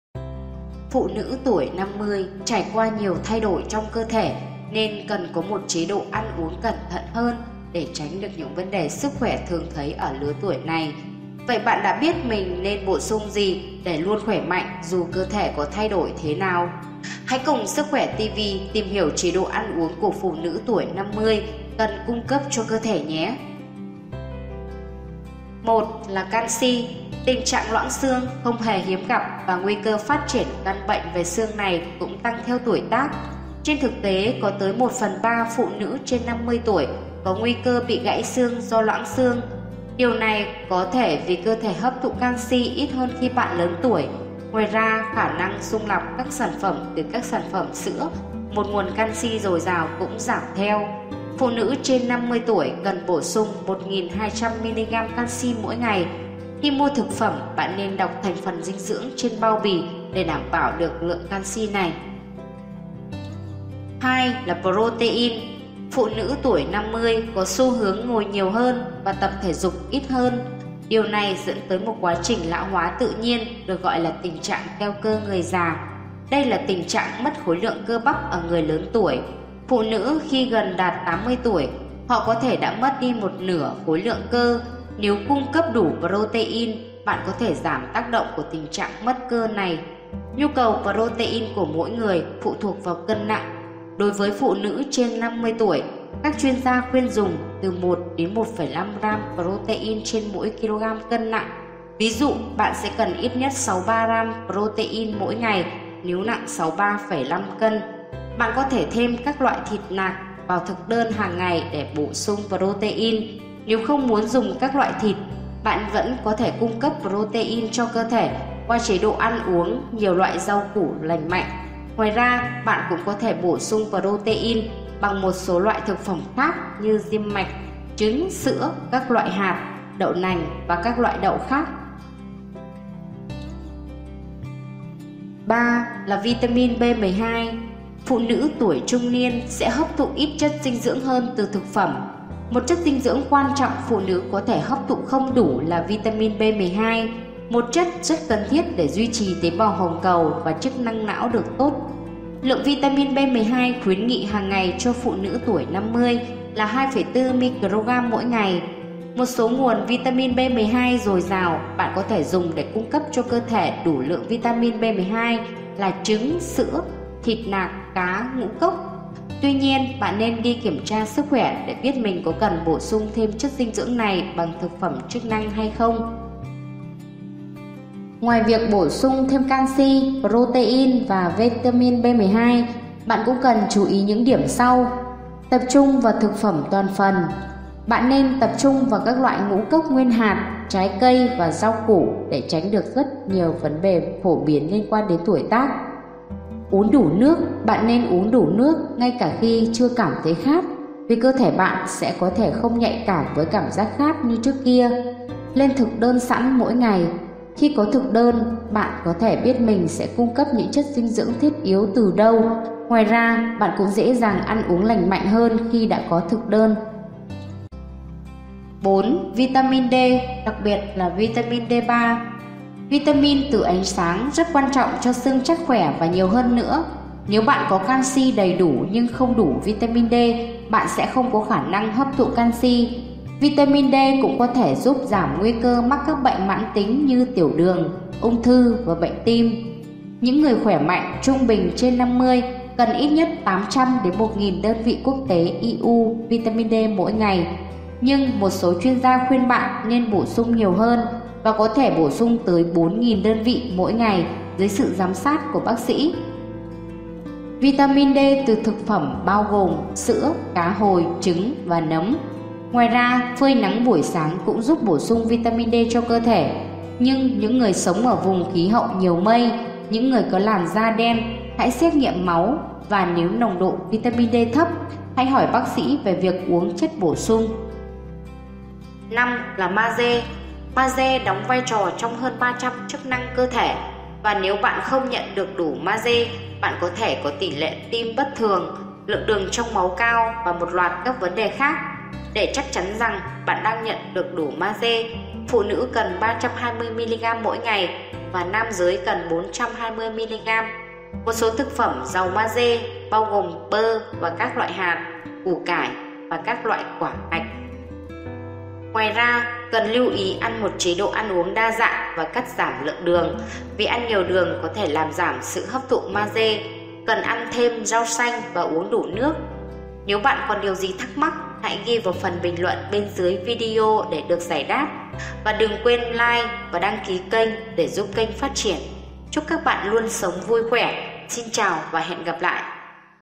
Ở Tuổi 50 Ngoài Collagen Hãy Bổ Sung Thêm Những Thành Phần Dinh Dưỡng Sau Để Cơ Thể Luôn Khỏe Mạn
Ở Tuổi 50 Ngoài Collagen Hãy Bổ Sung Thêm Những Thành Phần Dinh Dưỡng Sau Để Cơ Thể Luôn Khỏe Mạnh